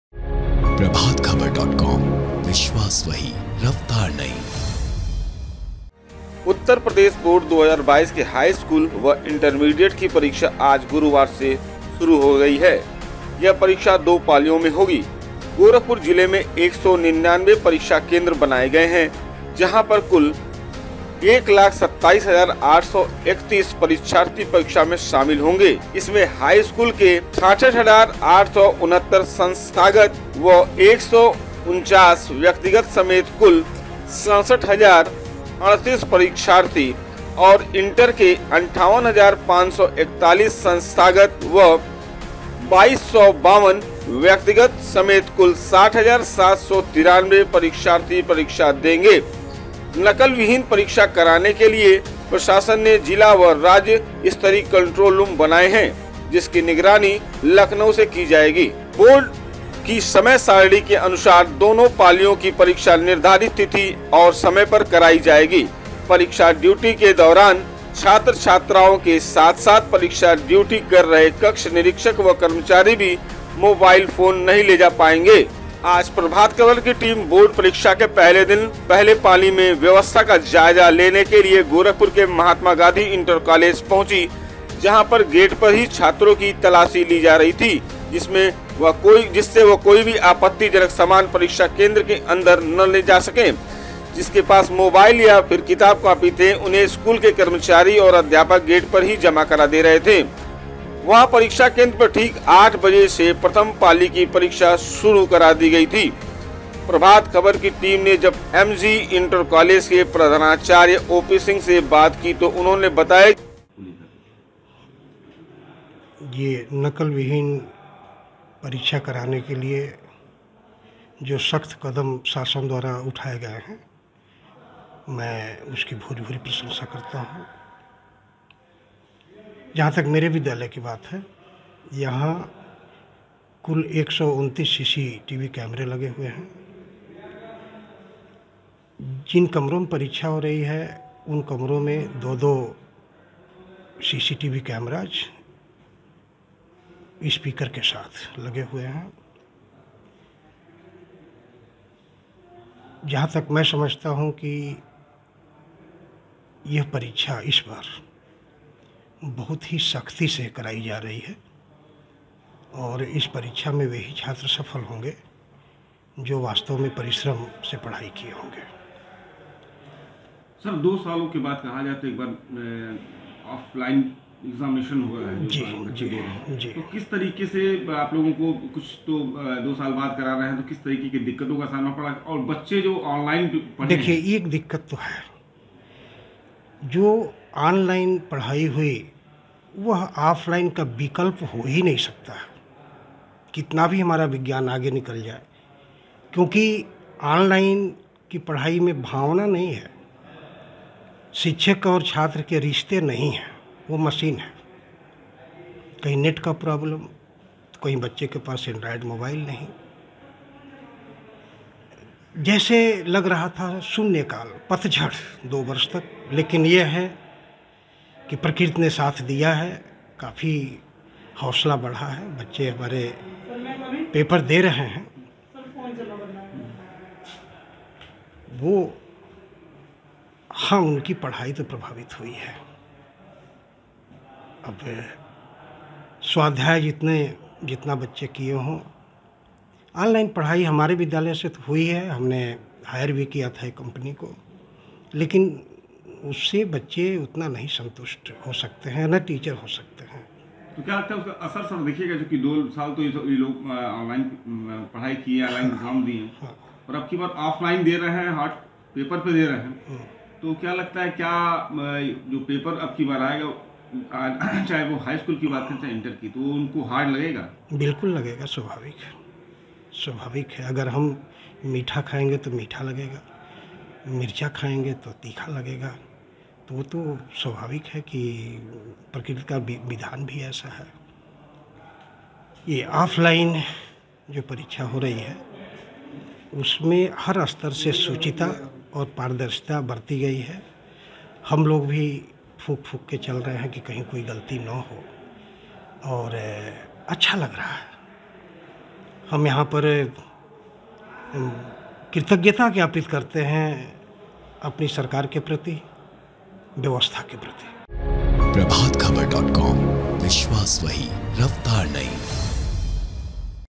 UP Board Exam 2022: गोरखपुर में बोर्ड परीक्षाएं शुरू दो पारियों में होंगी परीक्षाएं Prabhat Khabar
गोरखपुर, उत्तर प्रदेश बोर्ड 2022 की हाईस्कूल व इंटरमीडिएट की परीक्षा आज गुरुवार से हो रही है यह परीक्षा दो पालियों में होगी ,गोरखपुर जिले में 199 परीक्षा केंद्र बनाए गए हैं जहां पर कुल 127831 परीक्षार्थी परीक्षा में शामिल होंगे, इसमें हाई स्कूल के 66869 संस्थागत वह 149 व्यक्तिगत समेत कुल 67038 परीक्षार्थी और इंटर के 58541 संस्थागत व 2252 व्यक्तिगत समेत कुल 60793 परीक्षार्थी परीक्षा देंगे, नकल विहीन परीक्षा कराने के लिए प्रशासन ने जिला व राज्य स्तरीय कंट्रोल रूम बनाए हैं जिसकी निगरानी लखनऊ से की जाएगी ।